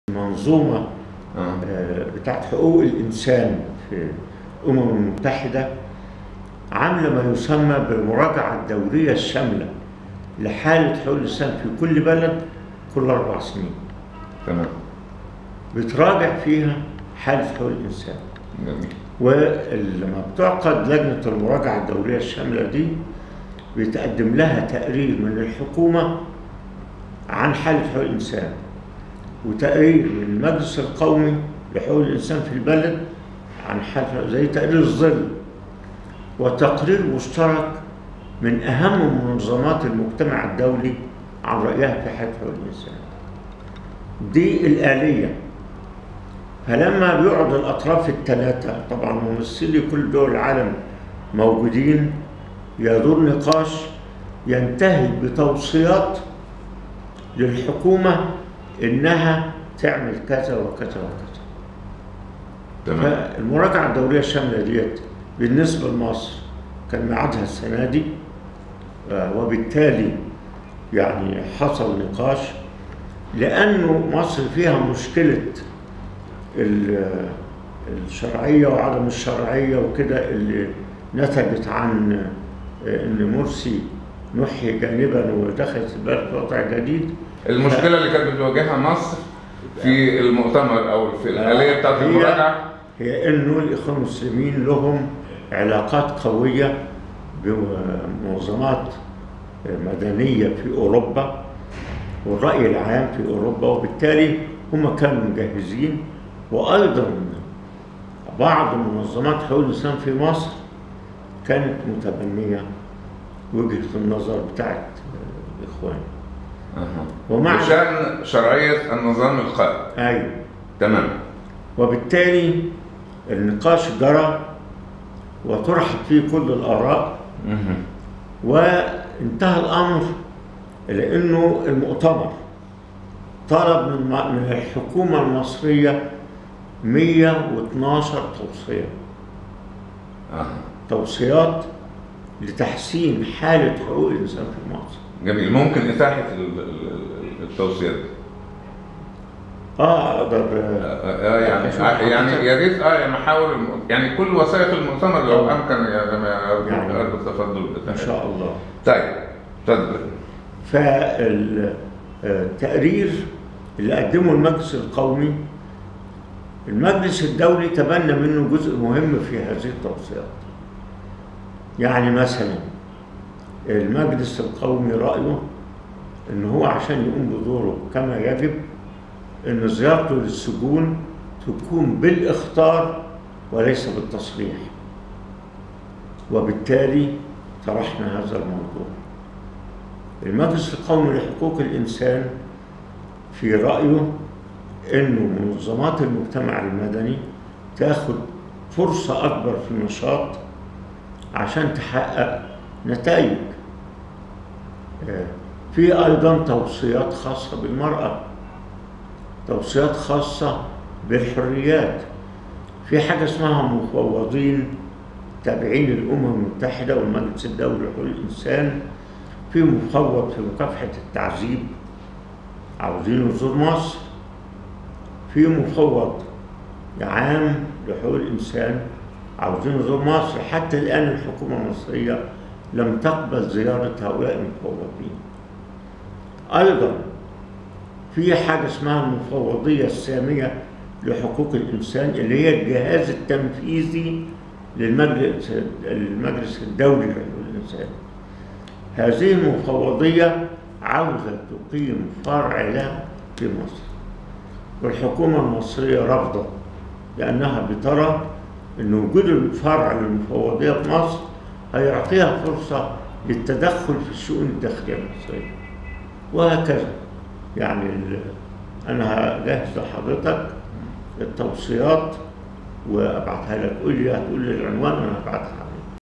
مراجعة دولية شاملة لشرعية النظام، و حالة حقوق الانسان في مصر (عبد الغفار شكر)
مراجعة شرعية النظام، و حالة حقوق الانسان في مصر، آمام المجلس الدولي
شرعية النظام في المجلس الدولي لحقوق الانسان، و مراجعته الدورية الشاملة للحالة في مصر
عبد الغفار شكر، نائب رئيس المجلس القومي لحقوق الانسان 11 يوليو 2016